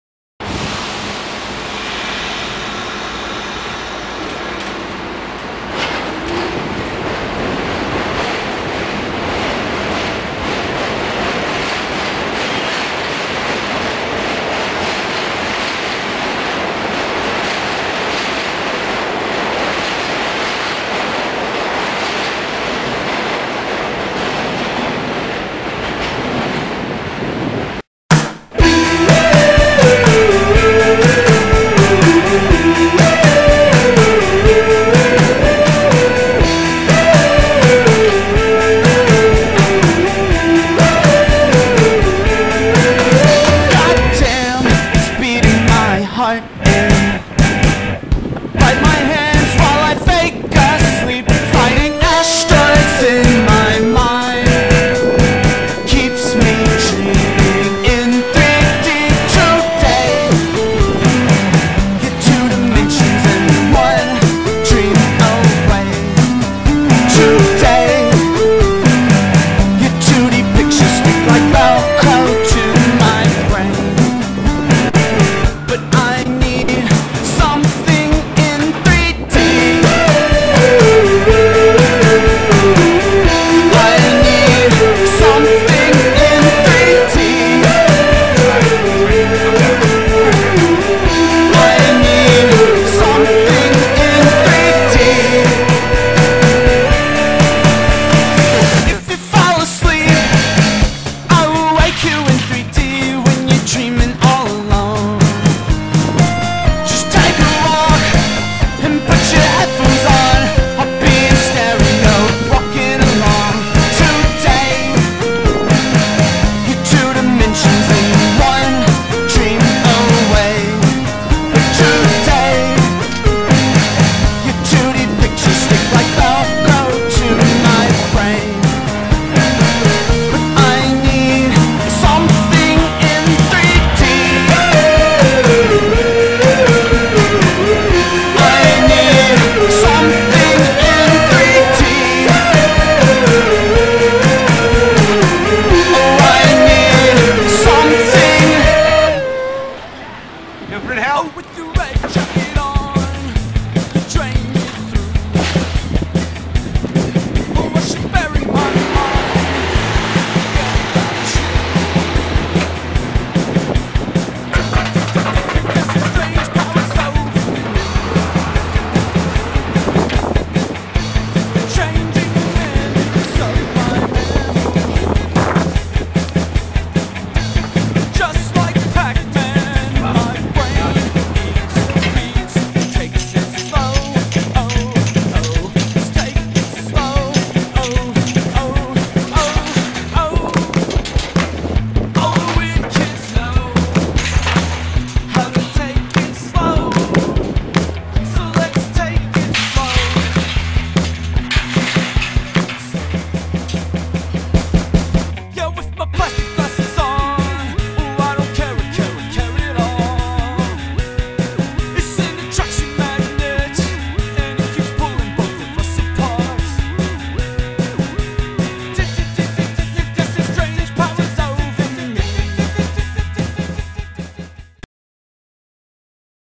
A Coney Island Day
Black Scorpion & Donny Vomit working hard on Coney Island's Sideshows by the Seashore.